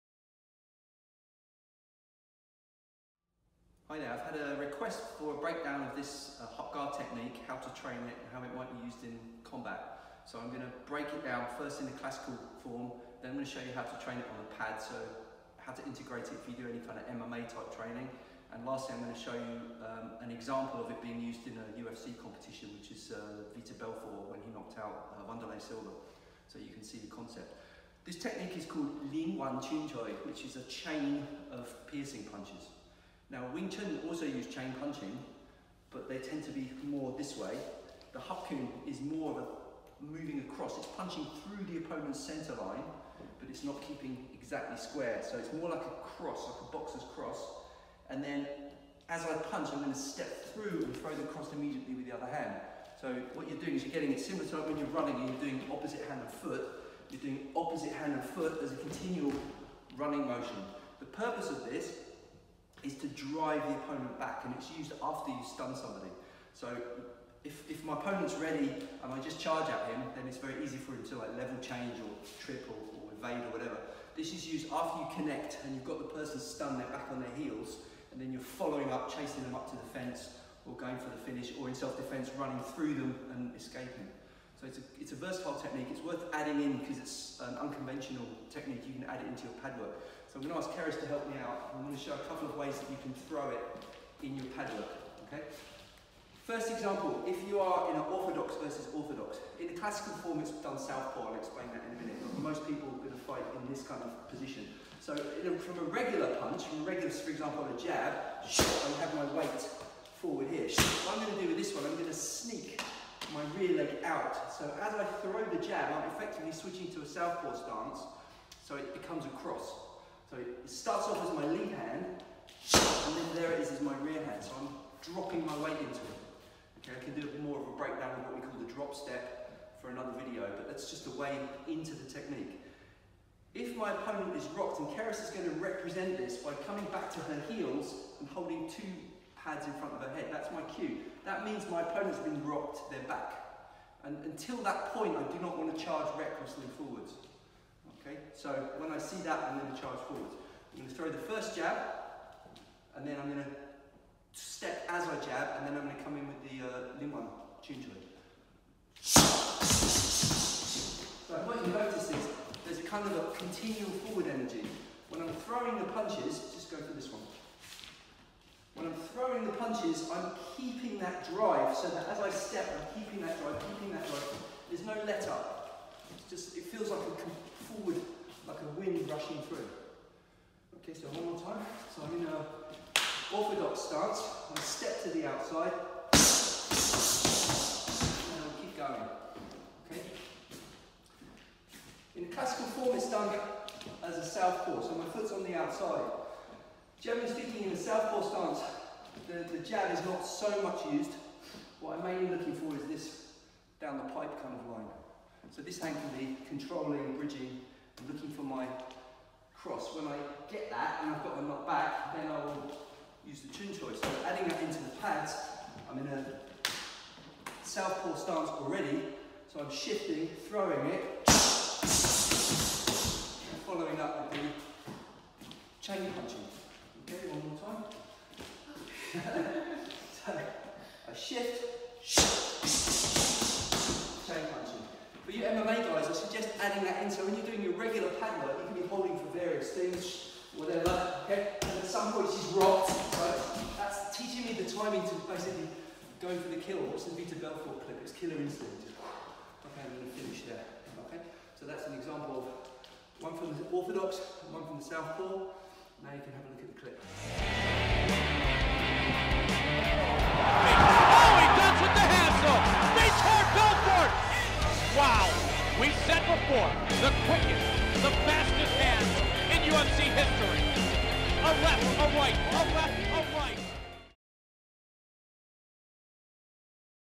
Hap Gar Chain Punching
This is an explanation of the Hap Gar Technique 'Lin wan Chyun Choi' (Chain Piercing Punches) and how to train them for Self Defence or MMA.